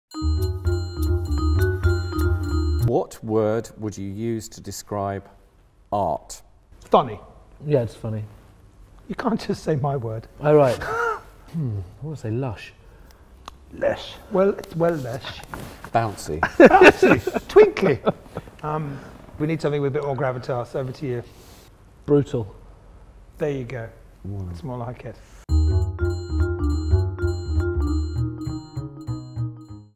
'ART' Q&A | What word would you use to describe 'Art'?
‘Apocalypse because of a white square…’

A global phenomenon celebrating its twentieth anniversary, Art returns to London for a limited run.